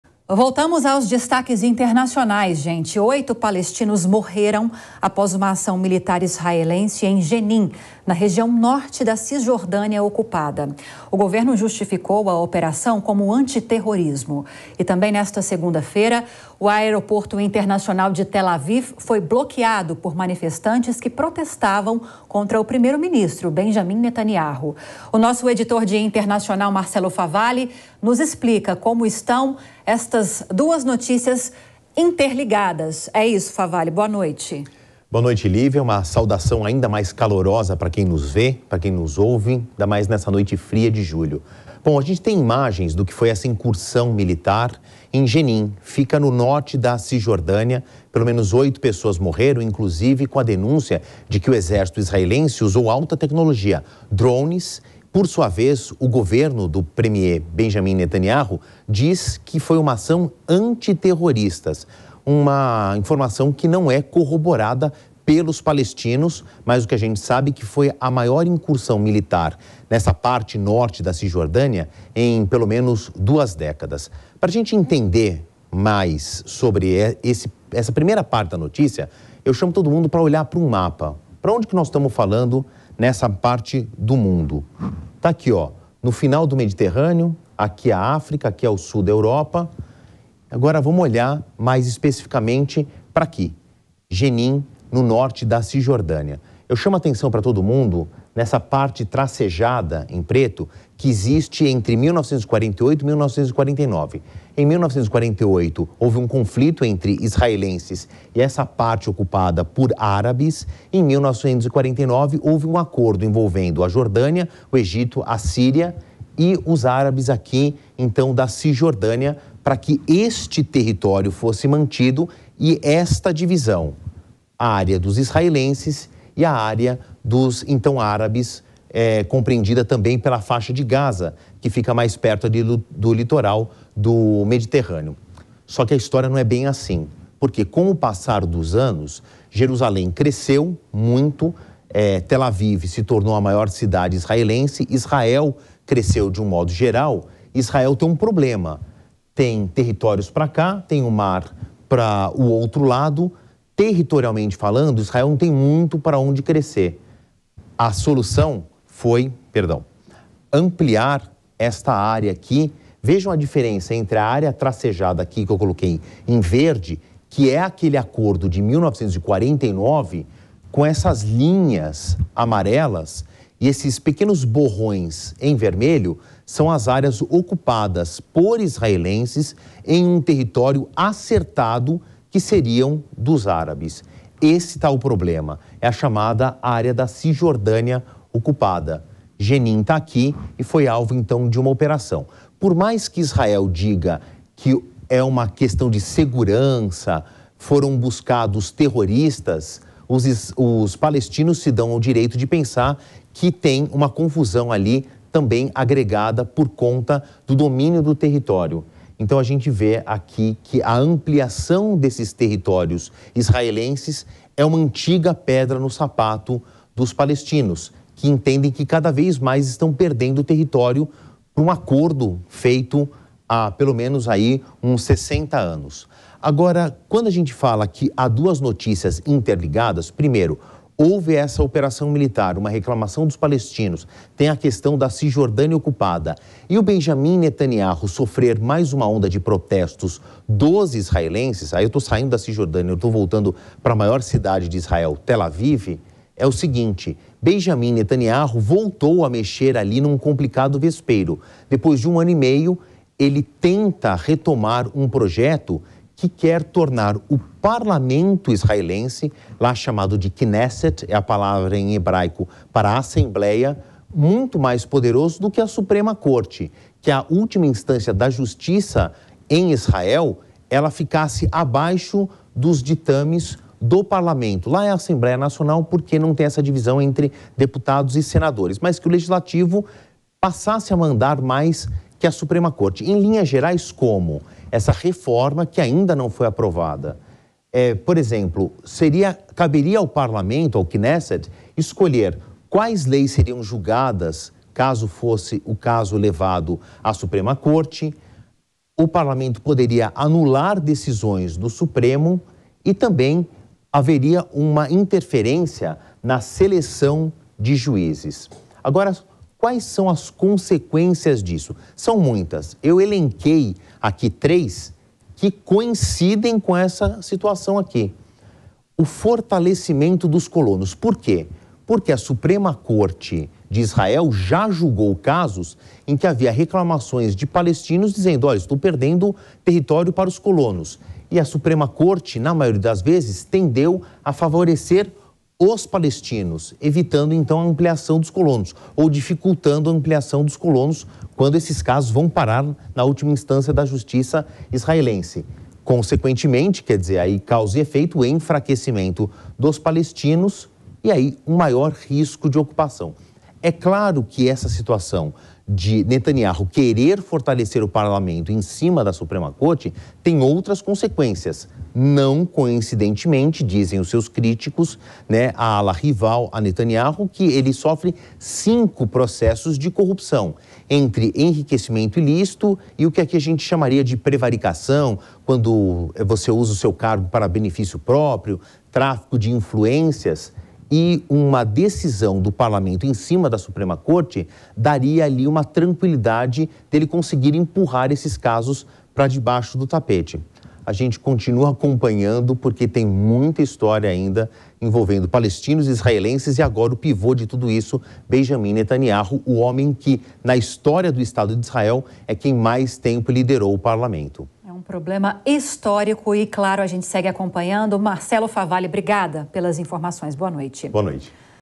Incursão militar de Israel à Cisjordânia deixa oito mortos; Marcelo Favalli analisa
Uma incursão militar de Israel à Cisjordânia realizada nesta segunda (03) deixou ao menos oito mortos. O governo de Benjamin Netanyahu falou em “ação antiterrorismo”. O premiê israelense fez uma proposta que pode diminuir o poder da Suprema Corte do país e das “superpoderes” à Assembleia Legislativa. Marcelo Favalli analisa.